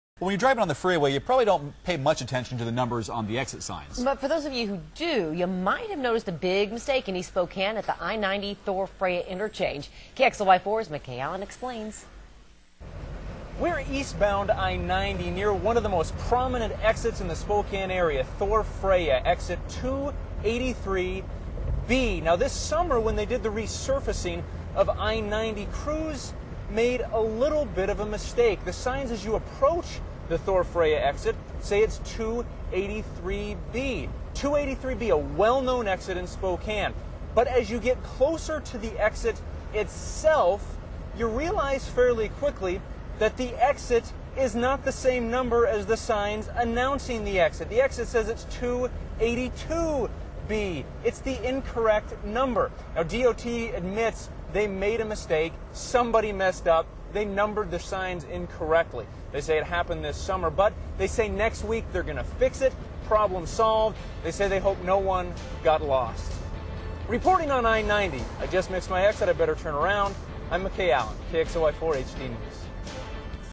Exit numbers messed up on the interstate?
A sign error temporarily causes confusion on Interstate 90. KXLY4's McKay Allen has more on the little slip-up.